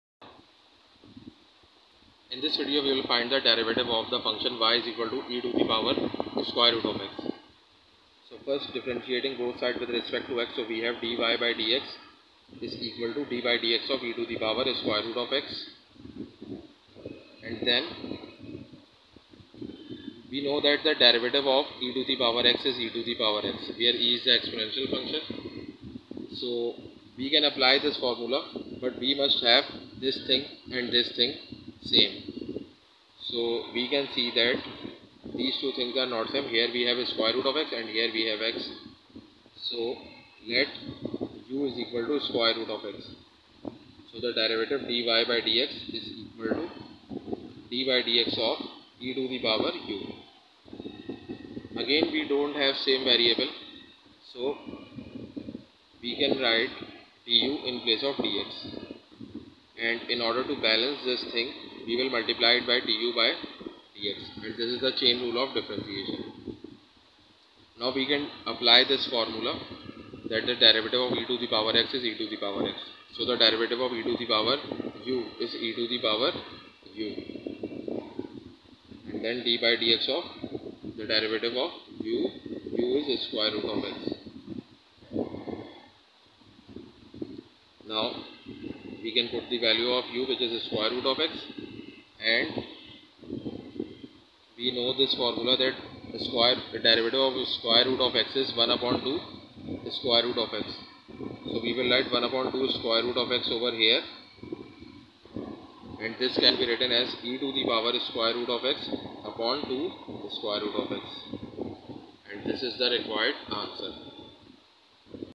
Derivative of e^√x || Differentiation of Exponential Function
In this video we shall learn how to find the derivative of an exponential function